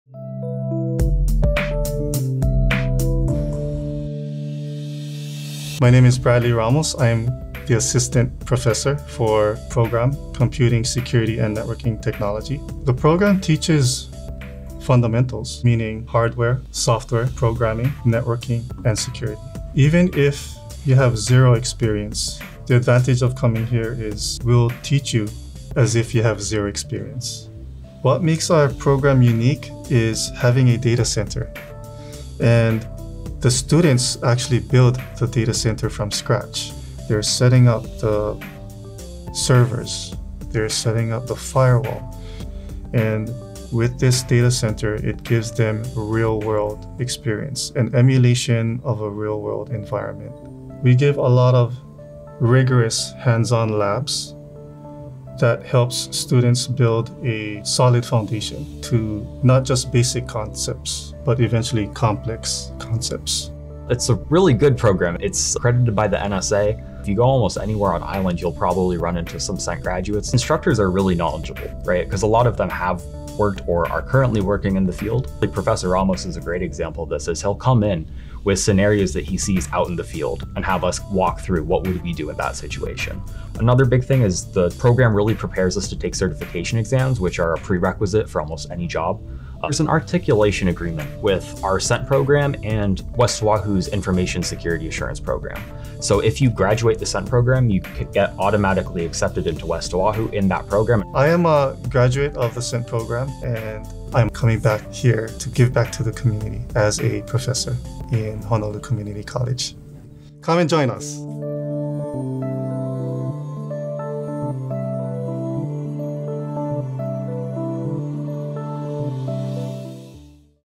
HonCC's Computing, Security & Networking Technology Program
Looking to break into the dynamic, highly in-demand field of computer technology? Meet your perfect program match: HonCC’s Computing, Security & Networking Technology (CSNT) program. Our classes are small, hands-on, and will provide you with a firm foundation in the basics of computers, networking, and information systems. Upon completion of the program, you’ll be ready for employment in the exciting world of Information and Communications Technology!

Our CSNT program has an Articulation Agreement with UH West Oʻahu. This means if you choose to transfer to UH West O‘ahu to earn a Bachelor of Applied Science (BAS) degree In Information Security and Assurance (ISA), the transition will be smooth and easy.

To learn more about this program or others like it, please visit the following link to our